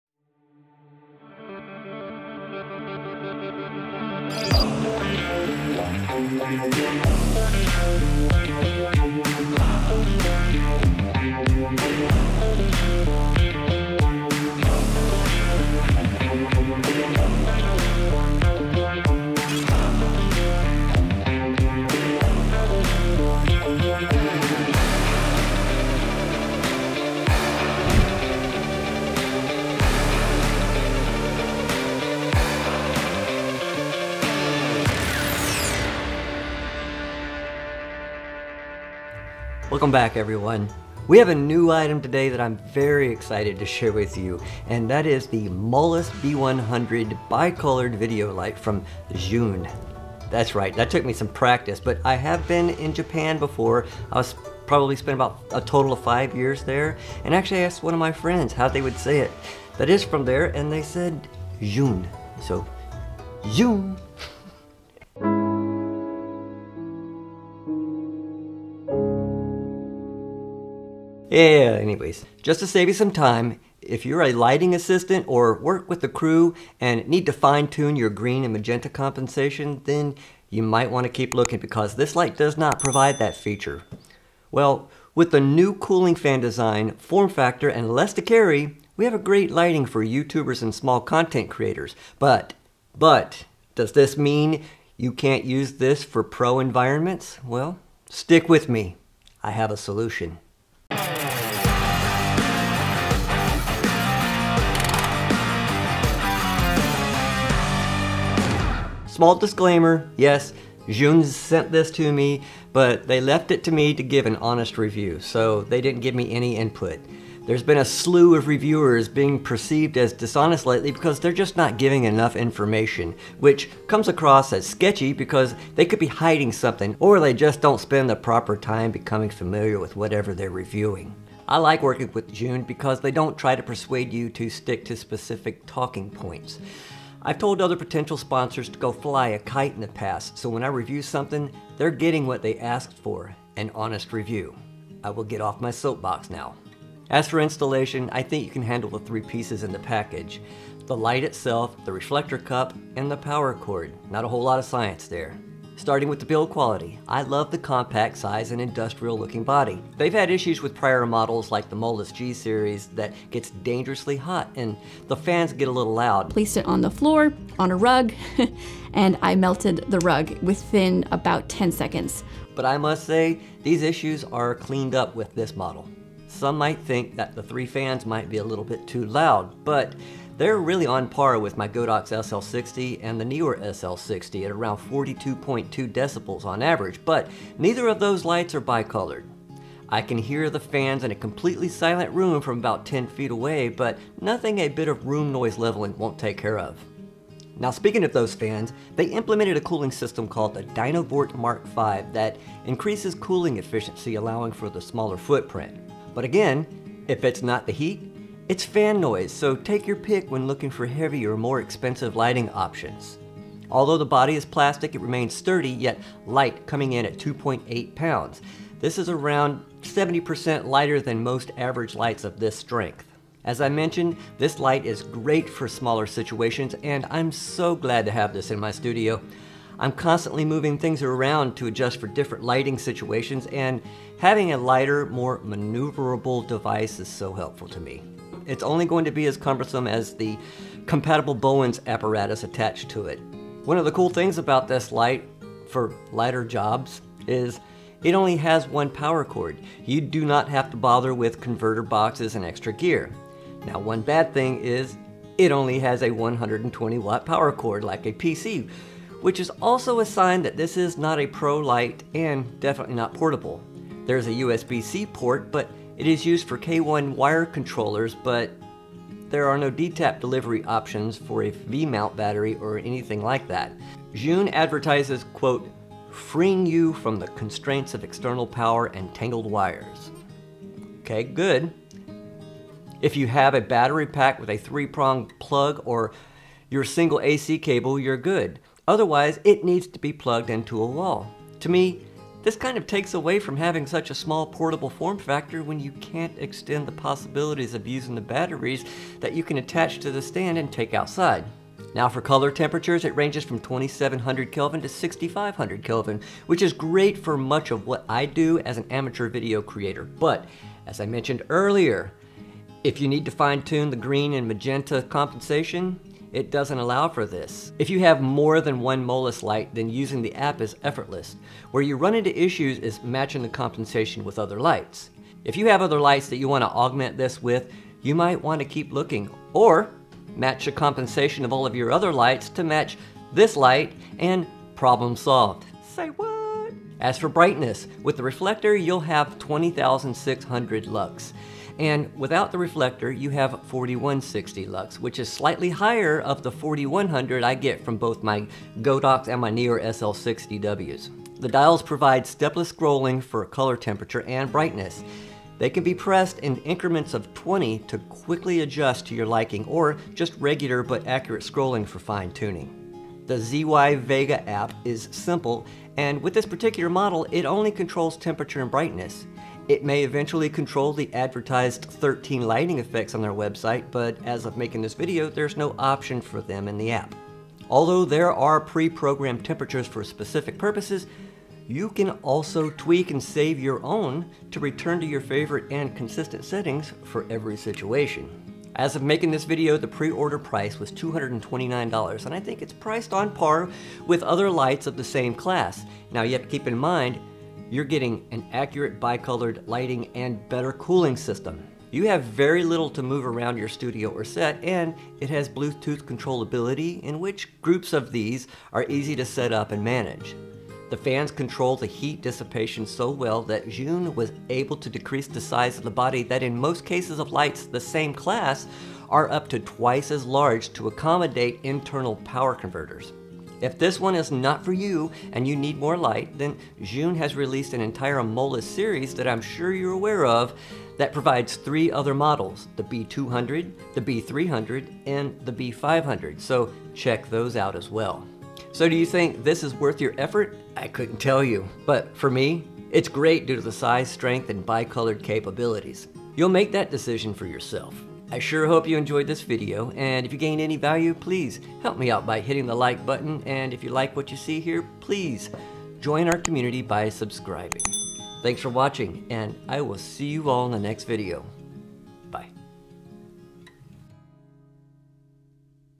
Set the Scene with New Zhiyun Molus B100 Video Light
Zhiyun's brand new Molus B100 COB Bi-Color Video lights are going to be very popular and convenient tools for all large or small content creators.  Color accuracy is achieved  by turning a dial, using an app or using their K1 controller all while maintaining a very cool and quiet setting with the new DynaVort cooling technology. This one is very affordable at $229.00. Check out their entire brand new MOLUS series B100, B200, B300, and the B500.

0:00 Intro
1:57 Honesty/Whats in the Box
2:55 Quiet Fans
3:33 Build Quality
3:49 Convenience
4:34 Deal Breaker?
5:51 Controls
7:03 Entire Molus Series
7:16 Closer

Camera for this video -
Canon EOS R
Audio -
Comica VM30 Shotgun